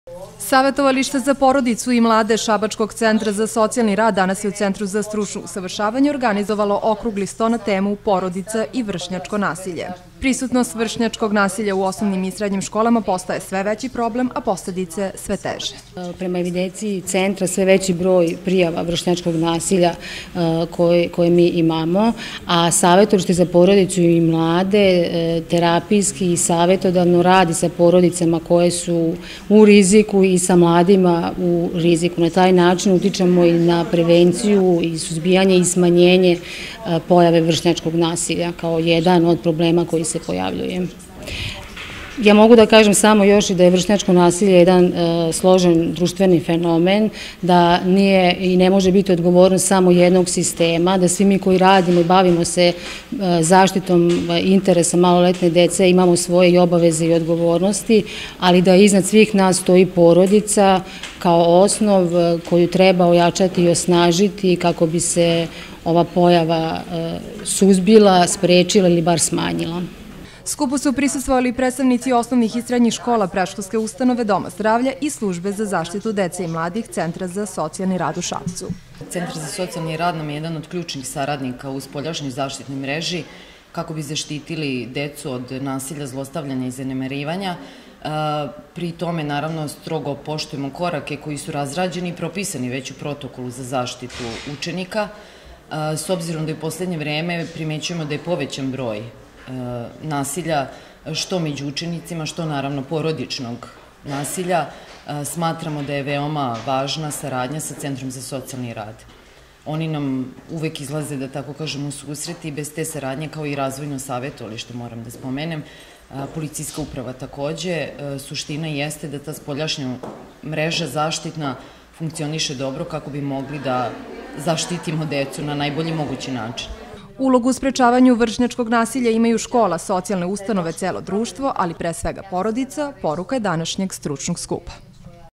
Okrugli sto "Porodica i vršnjačko nasilje"
U okviru obelezavanja Decje nedelje, Centar za socijalni rad  u Sapcu danas je organizovao okrugli sto u Centru za strucno usavrsavanje na temu „Porodica i vrsnjacko nasilje“. Skupu su prisustvovali i predstavnici sabackih osnovnih i srednjih skola, kao i Predskolske ustanove.